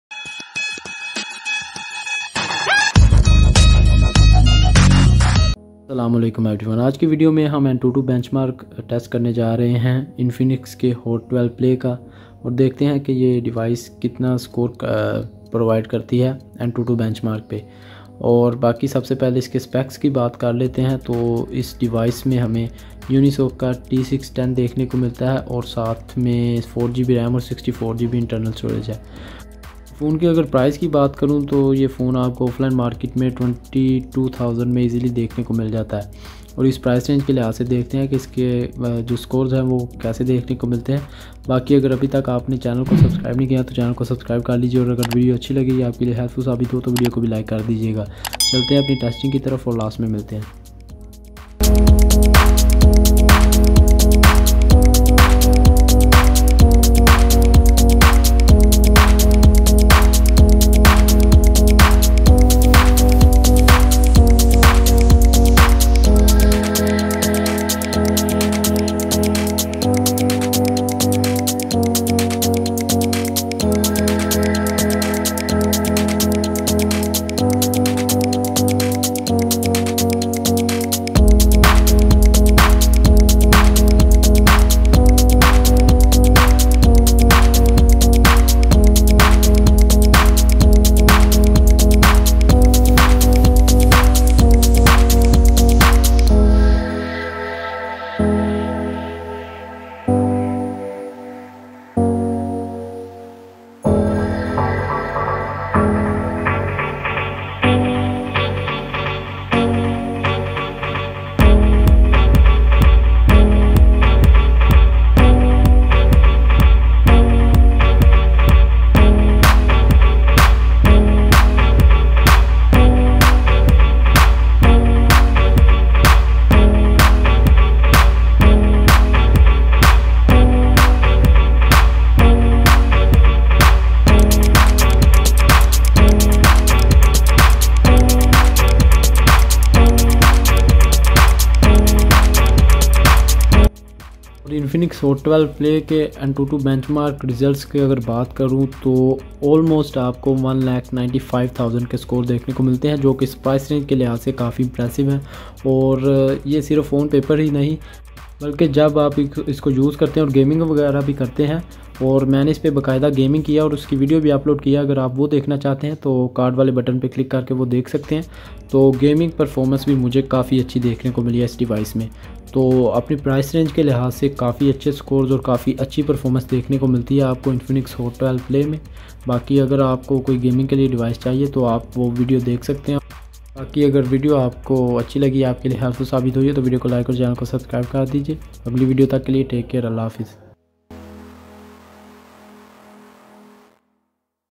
Infinix Hot 12 Play - ANTUTU BENCHMARK | Unisoc T610 antutu scores | Hot 12 Play Antutu Score!
Hello Friends
                 Today I am going to show you Infinix Hot 12 Play - ANTUTU BENCHMARK |  Unisoc T610 antutu scores | Hot 12 Play Antutu Score
         (The price of Hot 12 Play is 23,499 pkr and in this price
                 this is pne of the Best performance you can get)